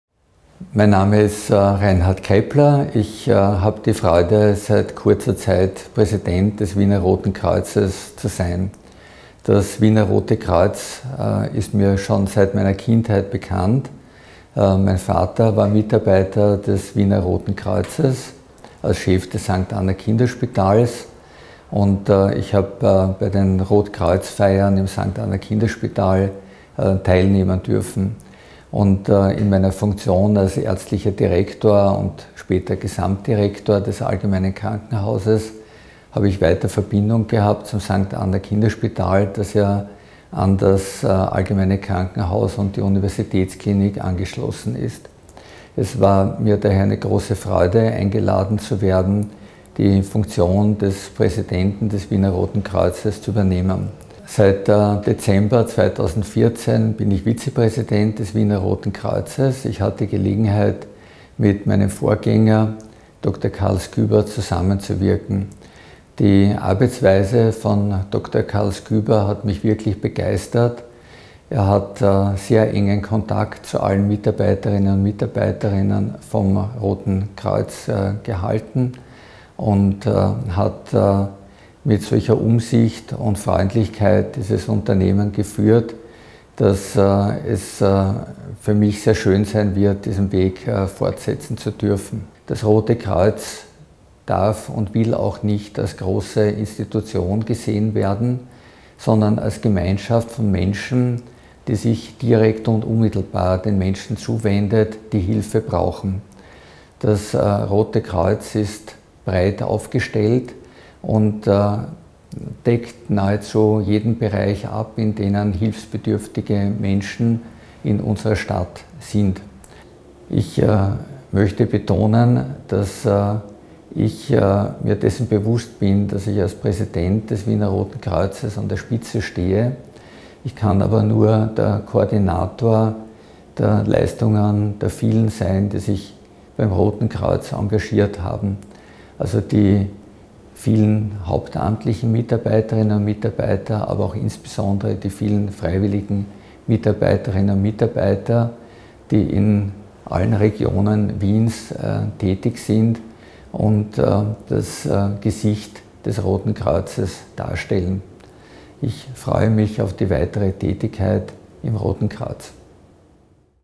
Videobotschaft Präsident Univ.-Prof. Dr. Reinhard Krepler - Kurzfassung
Antritts-Videobotschaft des neu gewählten Präsidenten des Wiener Roten Kreuzes.